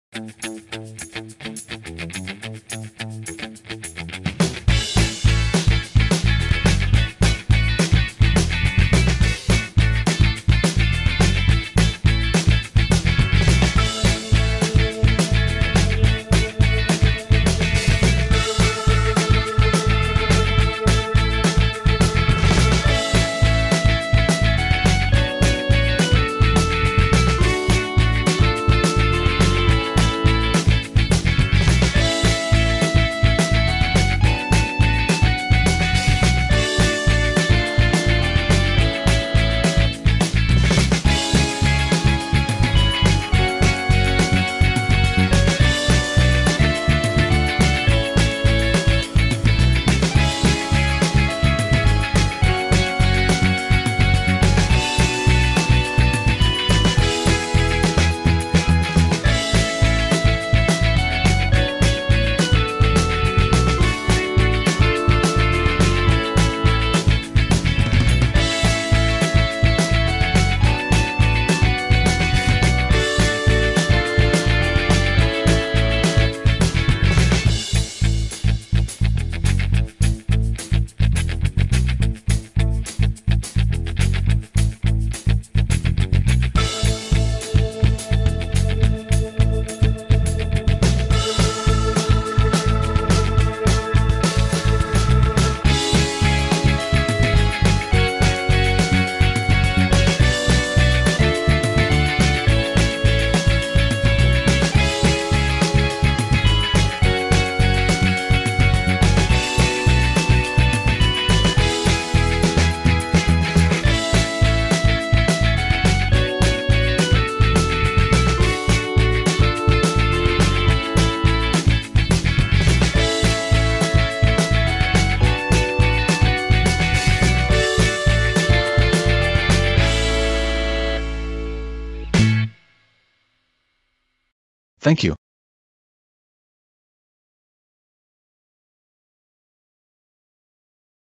Compilation Fortnite item Easy & Cute Speed Drawing #18｜Jolly Art NEGI
Today's Video is Compilation  Fortnite item Easy & Cute Speed Drawing #18 step-by-step drawing tutorial.
Jolly Art NEGI is easy drawing channel.
Thank you (^_^)